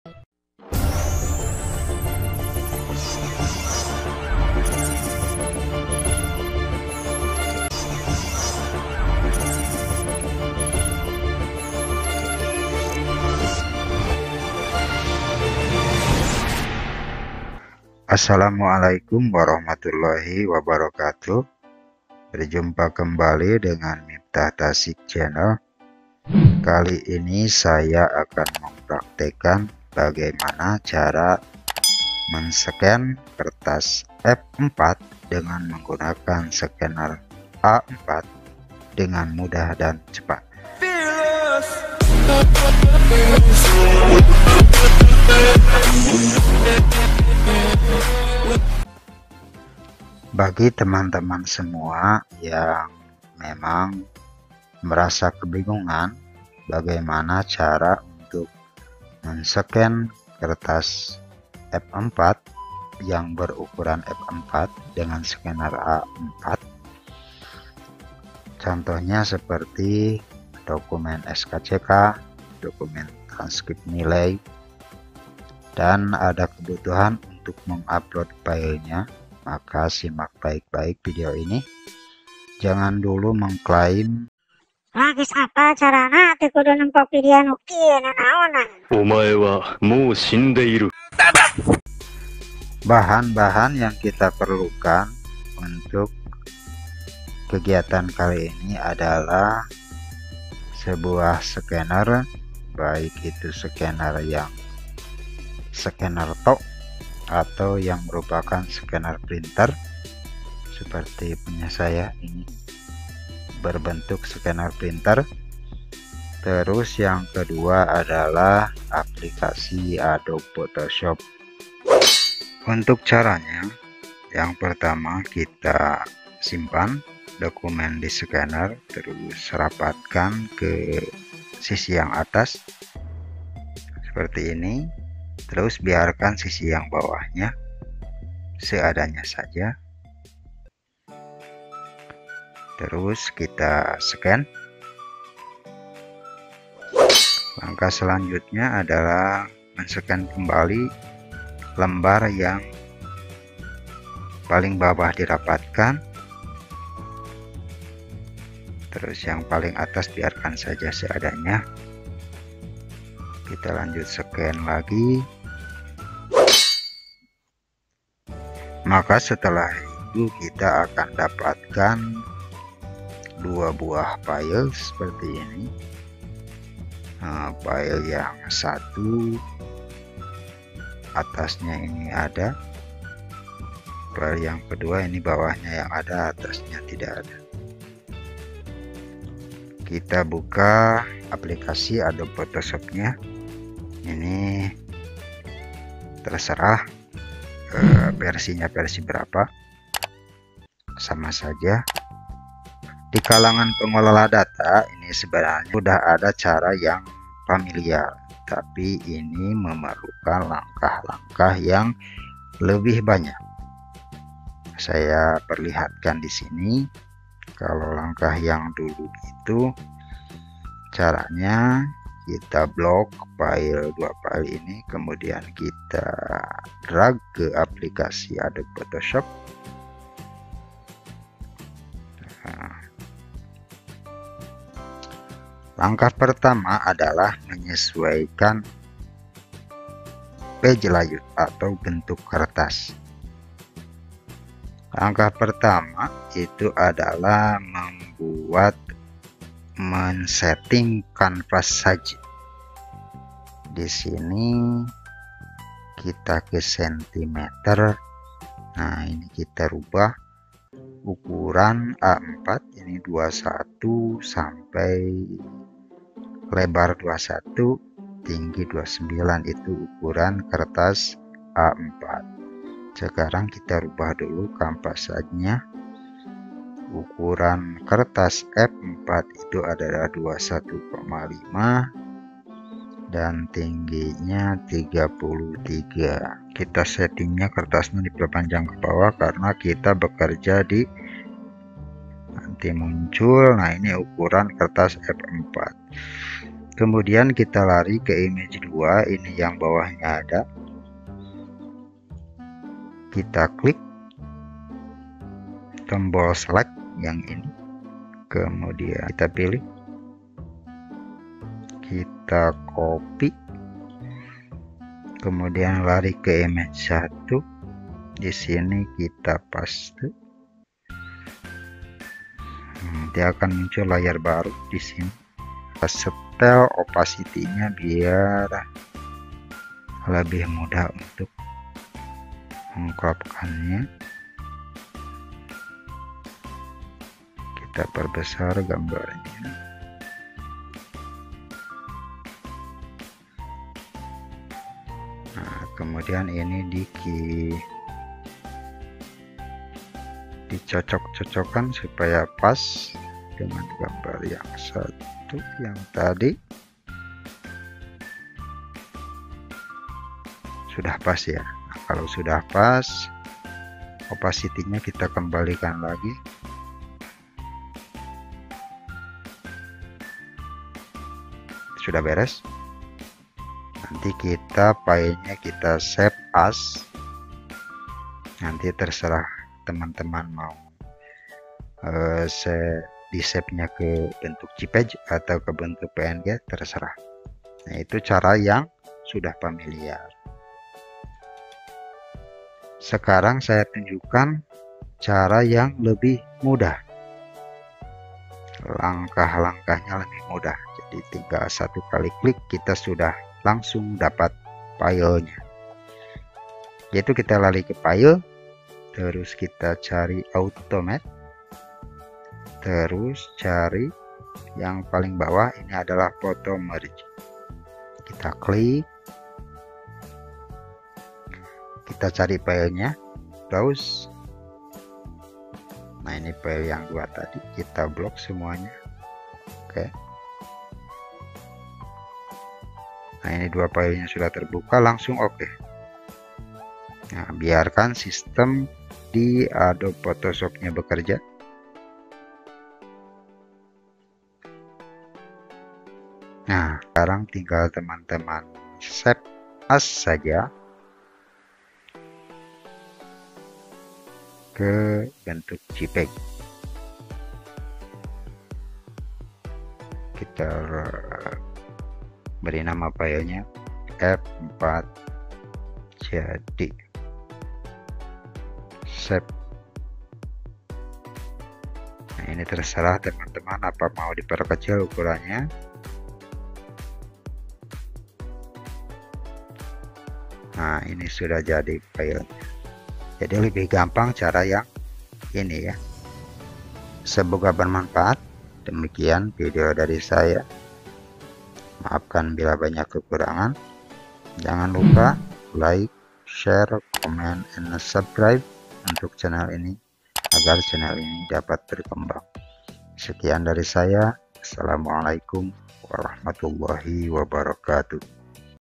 CARA SCAN KERTAS F4 di SCANNER A4 dengan CEPAT DAN MUDAH
Video berikut ini akan menjelaskan tentang bagaimana caranya scan kertas F4 di Scanner atau printer A4 dengan cepat, mudah, dan rapih tentunya, untuk lebih jelasnya silahkan tonton video berikut ini

Misalkan kalian ingin menscan SKCK tentu kesulitan untuk melakukannya, karena ukurannya F4.
Dan saya akan memberitahu cara mengatasi nya tanpa harus pakai alat scan tambahan/scanner khusus

00:00 Intro
01:33 Alat dan Cara
03:27 Cara Yang Sudah Familiar
06:33 Cara Yang Cepat

supaya tidak ketinggalan video berikutnya